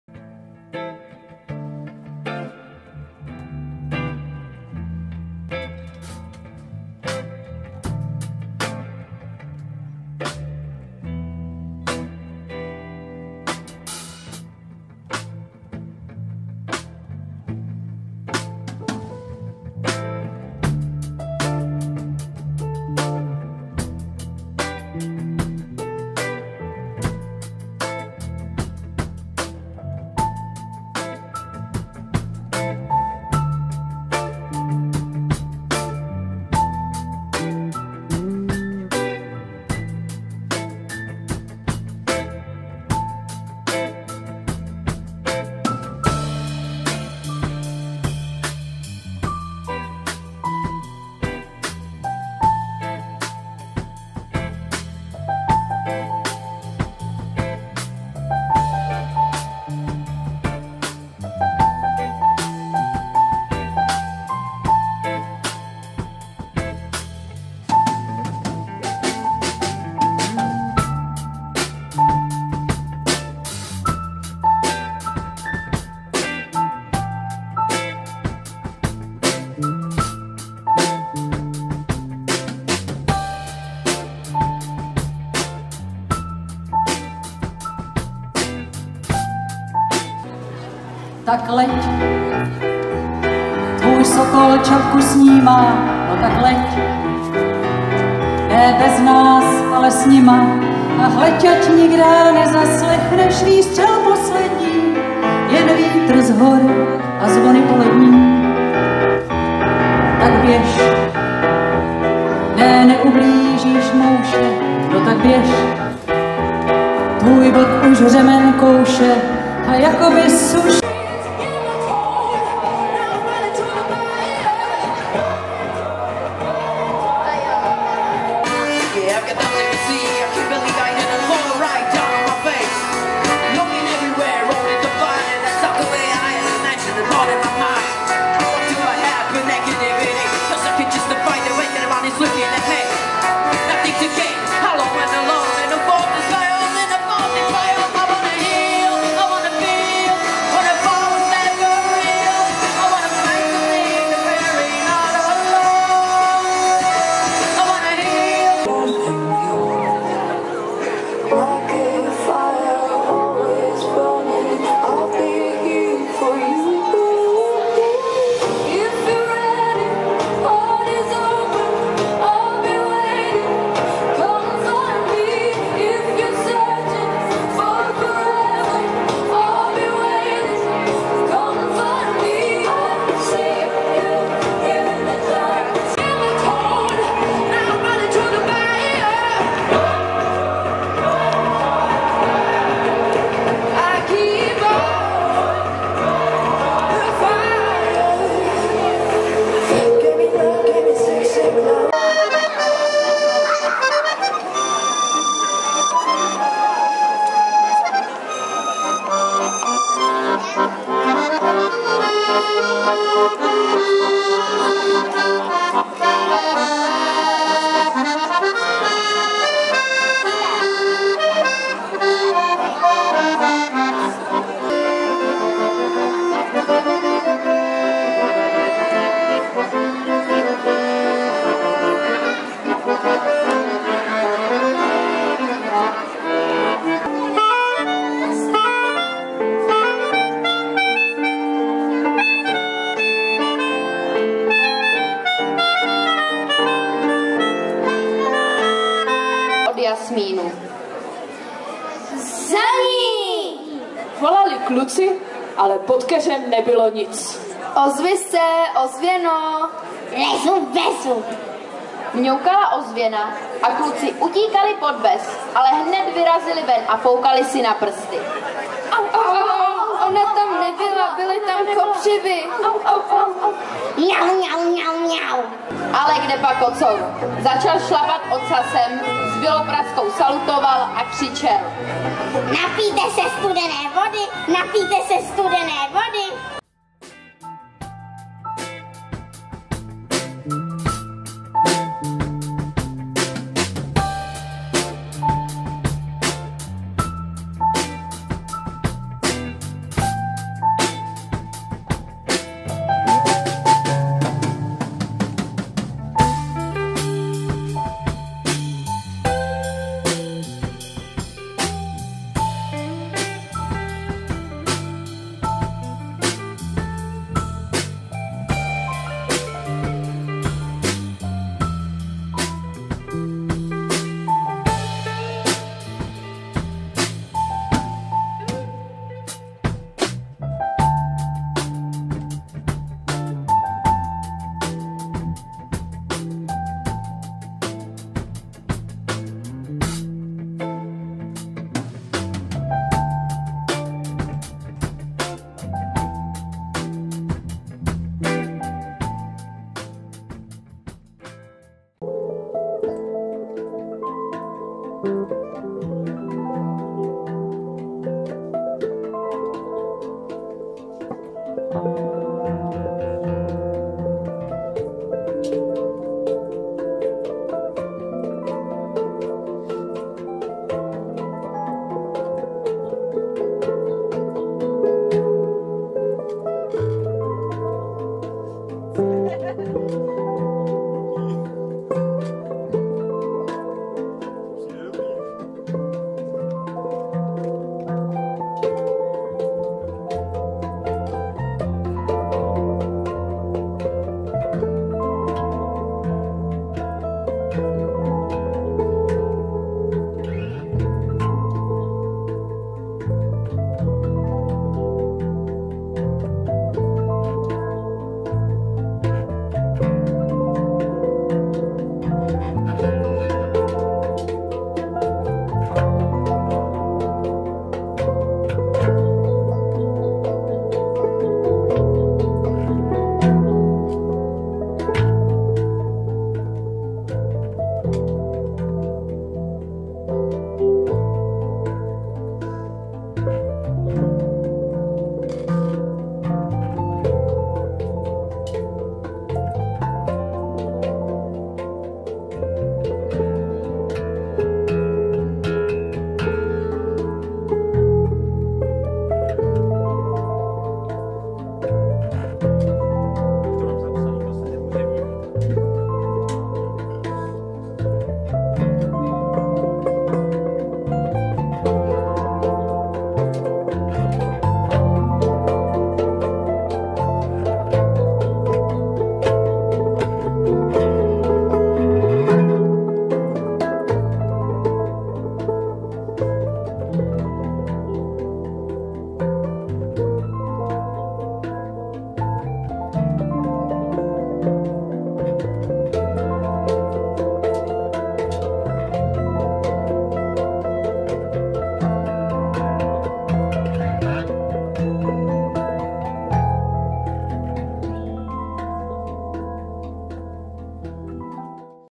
Arteradky - vernisáž 5.9.2021
Záznam z vernisáže Arteradky, která se konala v prostorách dobřichovického zámku v neděli 5.9.2021.

Doprovodná hudba Leamon.